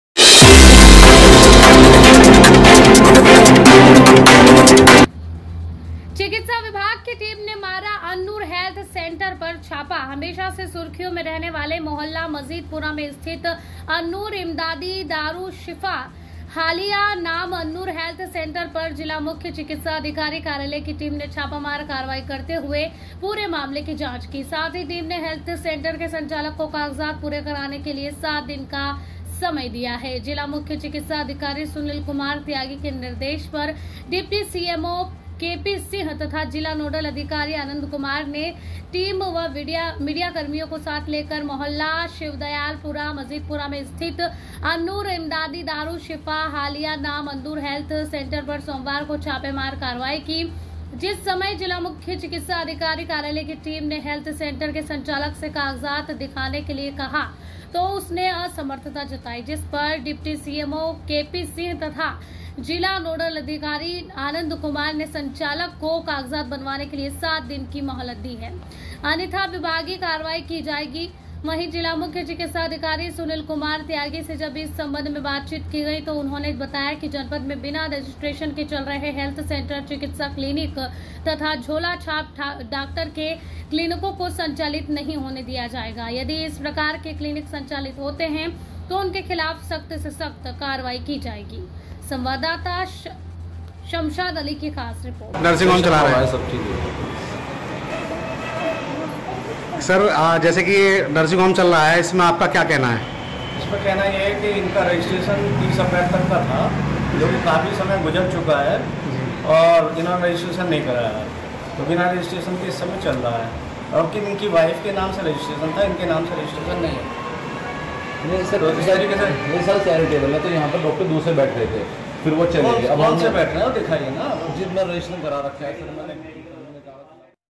चिकित्सा विभाग की टीम ने मारा अन्नूर हैल्थ सेंटर पर छापा
हापुड़ : हमेशा से सुर्खियों में रहने वाले मोहल्ला मजीदपुरा में स्थित अन्नूर इमदादी दारू शिफा हालिया नाम अन्नूर हैल्थ सेंटर पर जिला मुख्य चिकित्सा अधिकारी कार्यालय की टीम ने छापामार कार्यवाही करते हुए पूरे मामले की जांच की। साथ ही टीम ने हैल्थ सेंटर के संचालक को कागजात पूरे कराने के लिए सात दिन का समय दिया।
जिला मुख्य चिकित्सा अधिकारी सुनील कुमार त्यागी के निर्देश पर डिप्टी सीएमओ के पी सिंह तथा जिला नोडल अधिकारी आनंद कुमार ने टीम व मिडियाकर्मियों को साथ लेकर मोहल्ला शिवदयालपुरा मजीदपुरा में स्थित अन्नूर इमदादी दारू शिफा हालिया नाम अन्नूर हैल्थ सेंटर पर सोमवार को छापामार कार्यवाही की। जिस समय जिला मुख्य चिकित्सा अधिकारी कार्यालय की
 टीम ने हैल्थ सेंटर के संचालक से कागजात दिखाने के लिए कहा तो उसने असमर्थता जताई। जिस पर डिप्टी सीएमओ के पी सिंह तथा जिला नोडल अधिकारी आनंद कुमार ने संचालक को कागजात बनवाने के लिए सात दिन का समय दिया। अन्यथा विभागीय कार्यवाही की जाएगी।
वहीं जिला मुख्य चिकित्सा अधिकारी सुनील कुमार त्यागी से जब इस संबंध में बातचीत की गई तो उन्होने बताया कि जनपद में बिना रजिट्रेशन के चल रहे हैल्थ सेंटर चिकित्सा क्लीनिक तथा झोलाछाप डॉक्टर के क्लीनिको को संचालित नहीं होने दिया जाएगा। यदि इस प्रकार के क्लीनिक संचालित हैं तो उनके खिलाफ सख्त से सख्त कार्यवाही की जाएगी।

शमशाद अली की खास रिपोर्ट